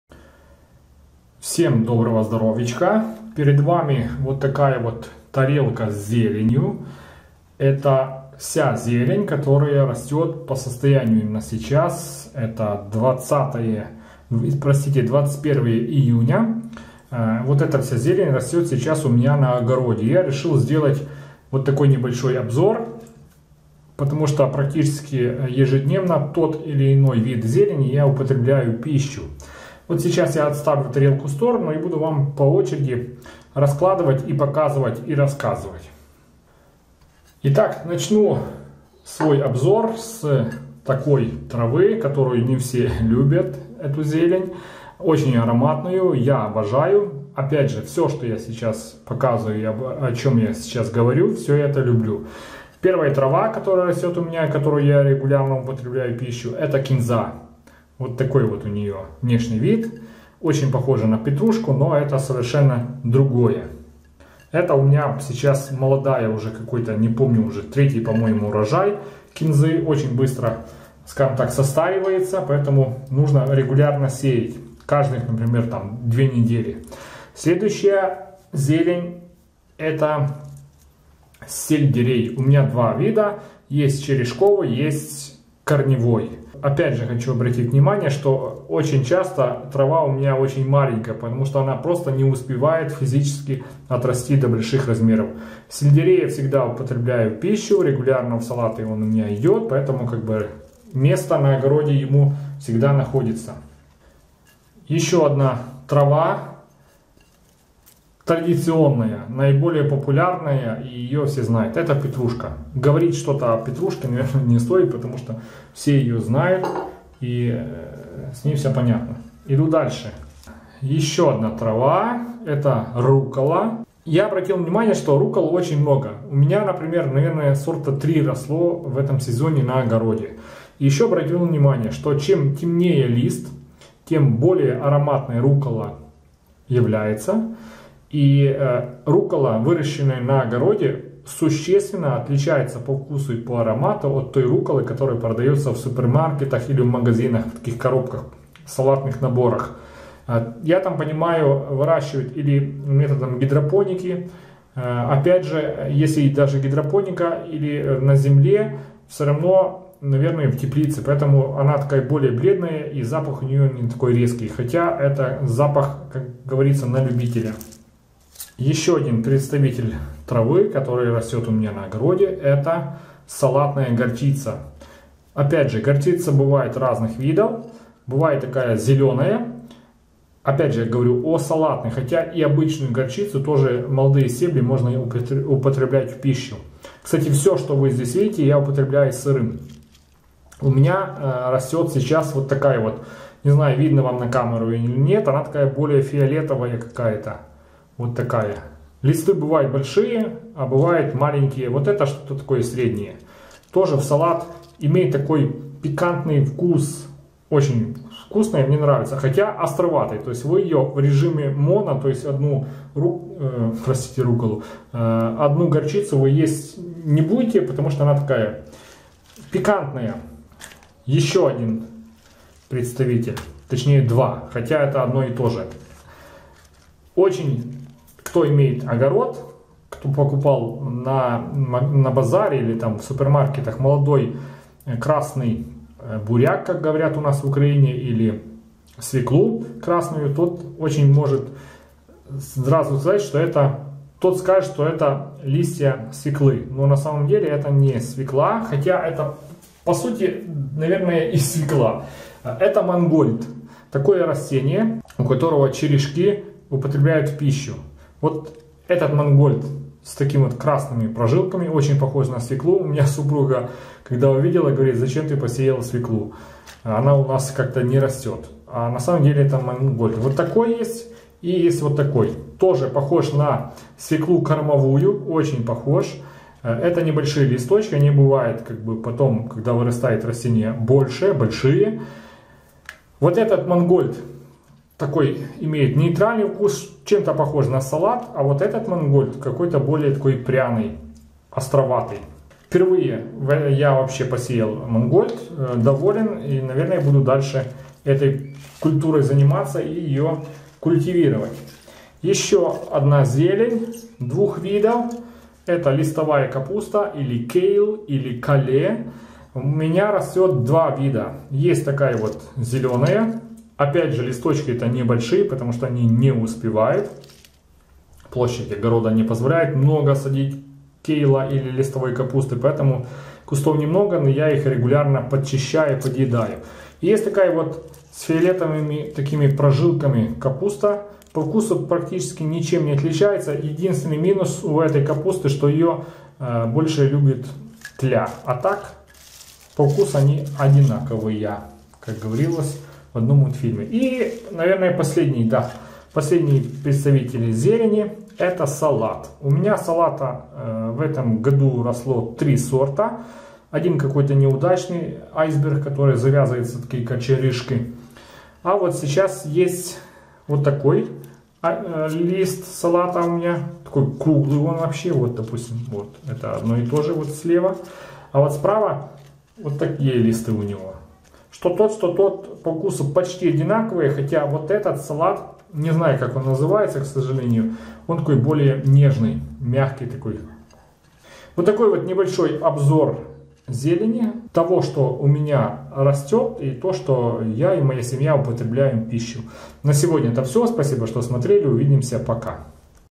Виды зелени растущей на моем огороде - обзор
В последние годы свежая зелень в моем рационе регулярно, кроме зимних месяцев. Петрушка, кинза, кейл (листовая капуста), мангольд, сельдерей, руккола, салат, салатная горчица, все это сейчас растет у меня на огороде и все это идет в свежем виде в различные салаты, а также в соусы.
Помочь каналу:
WebMoney кошельки:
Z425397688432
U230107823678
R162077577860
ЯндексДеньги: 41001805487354